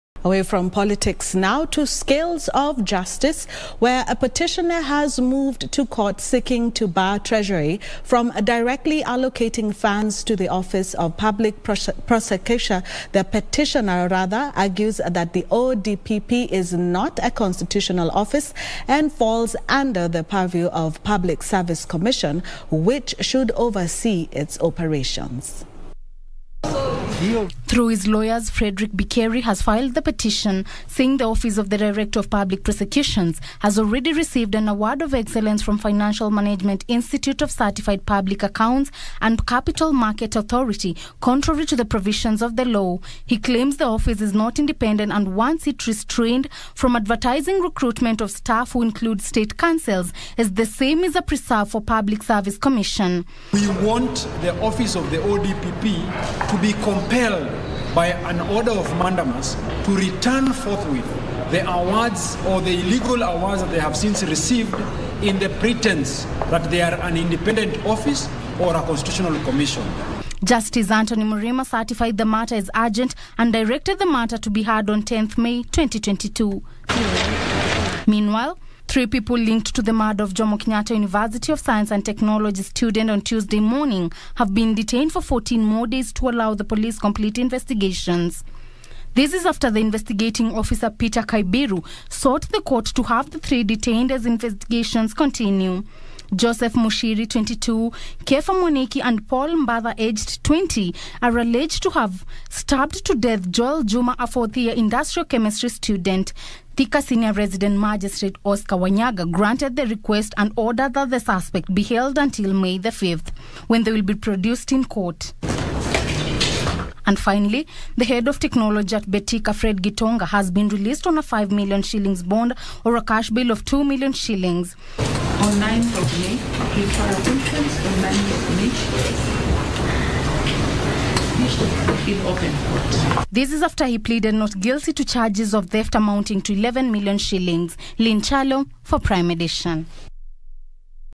Petitioner moves to Court on the constitutionality of ODPP office
A petitioner has moved to court seeking to bar Treasury from directly allocated funds to the Office of Public Prosecution. The petitioner claims the office is not a constitutional office and falls under the purview of the Public Service Commission which should oversee it operations.